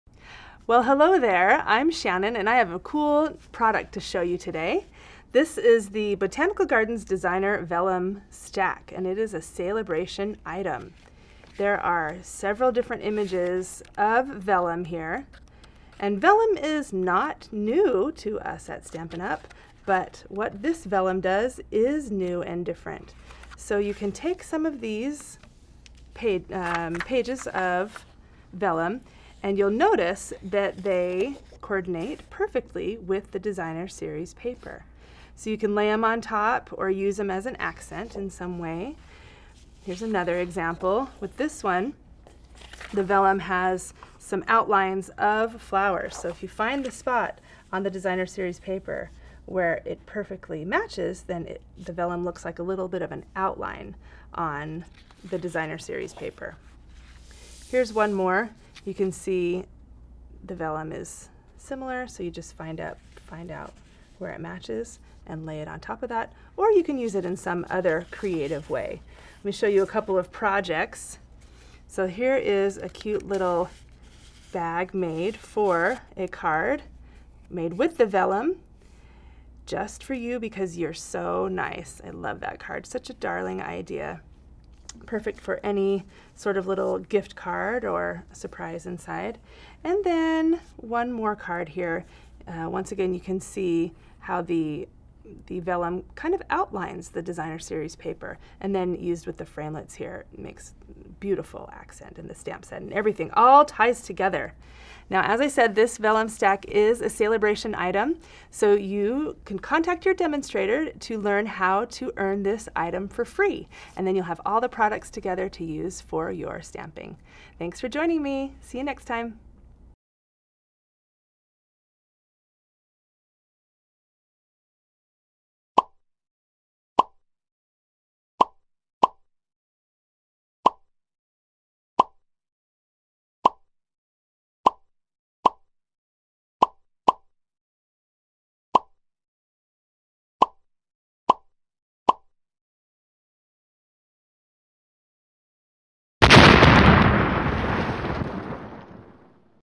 Botanical Gardens Designer Vellum Stack by Stampin’ Up!
Vellum sheets embossed with white printing make gorgeous accents, invitations, and overlays. Or layer it over your favorite cardstock or Designer Series Paper to add color. See what you can do with a simple but elegant stack of paper.

Want to get this product FREE? Contact your Stampin’ Up! demonstrator to find out how. If you don’t have a demonstrator, go to stampinup.com and choose Find a Demonstrator.